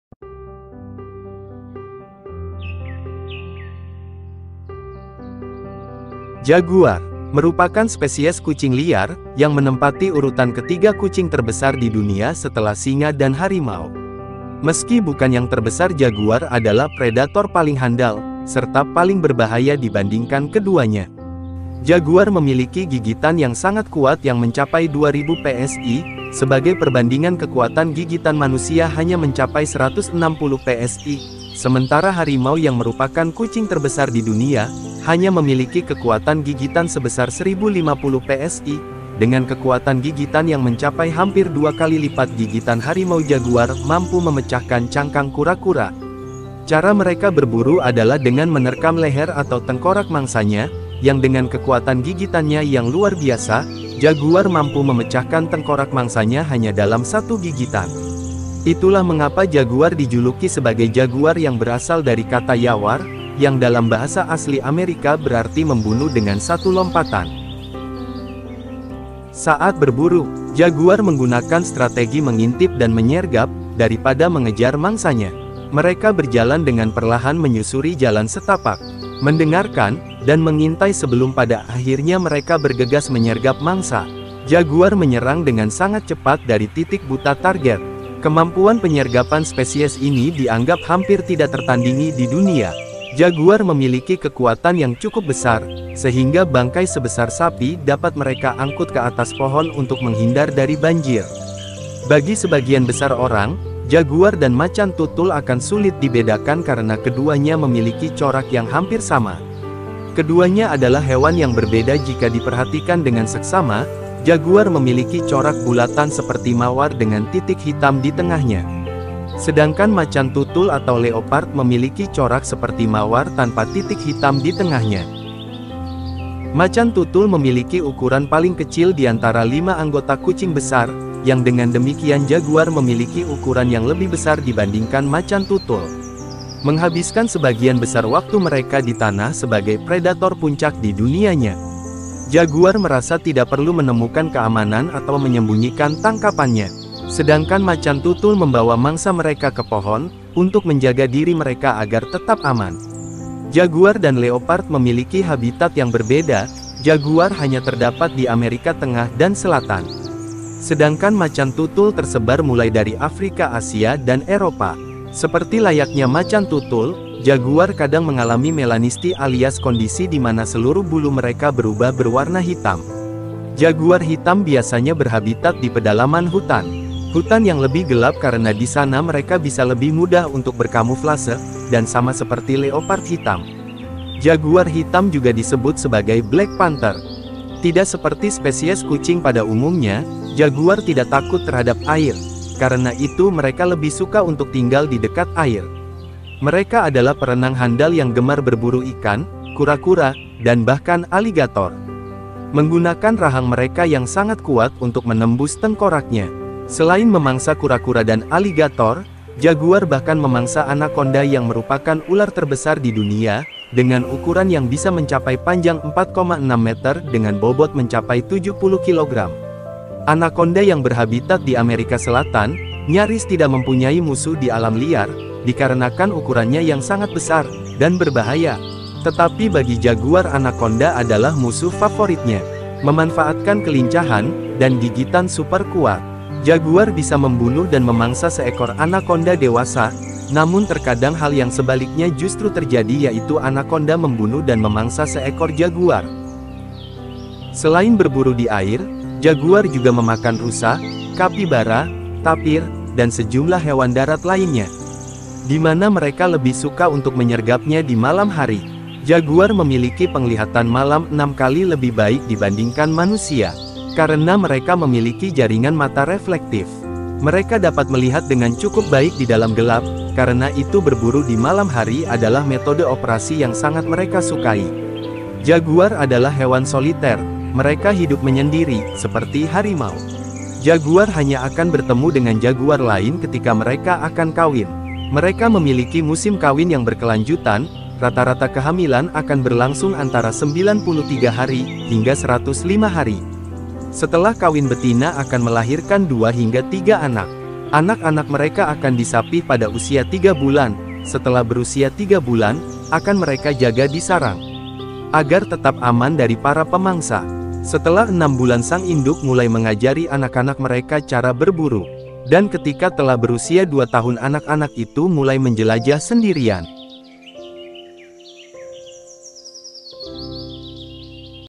Jaguar | Kucing Besar yang Mampu Bunuh Anakonda
Jaguar adalah hewan sejenis kucing besar yang tergolong dalam genus Panthera. Binatang ini merupakan kucing liar ketiga setelah harimau dan singa, dan juga merupakan kucing terbesar di belahan Barat. Binatang ini meliputi wilayah Meksiko utara, melintasi Amerika Tengah hingga ke Paraguay, Argentina utara dan selatan.